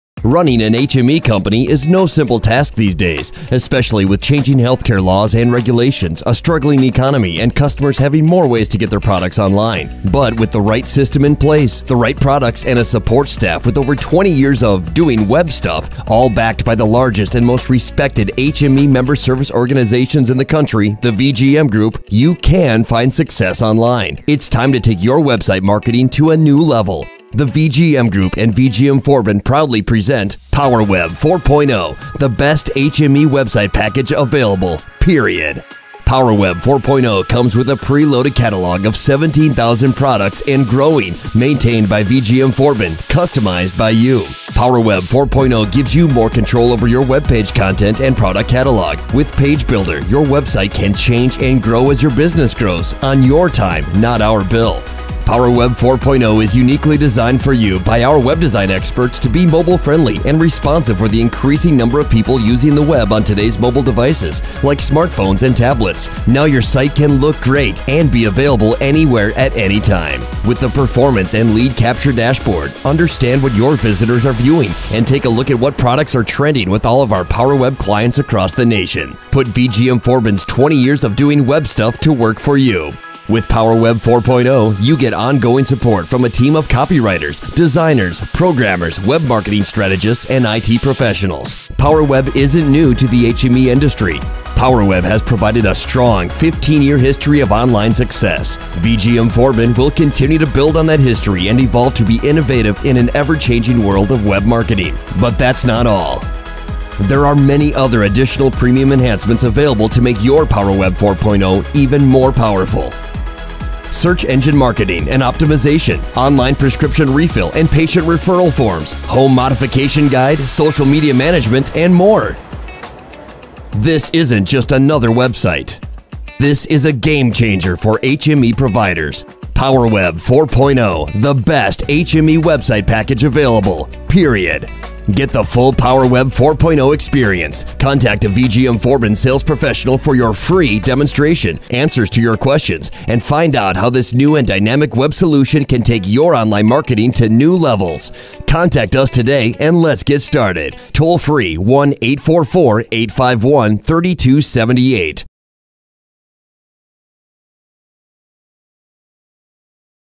PowerWeb 4.0
Our most popular web solution, PowerWeb serves the needs of home medical equipment suppliers of all sizes. Not only does it give them a web presence, but it also serves as a way to get potential business leads. There really is no better way to represent your HME business online.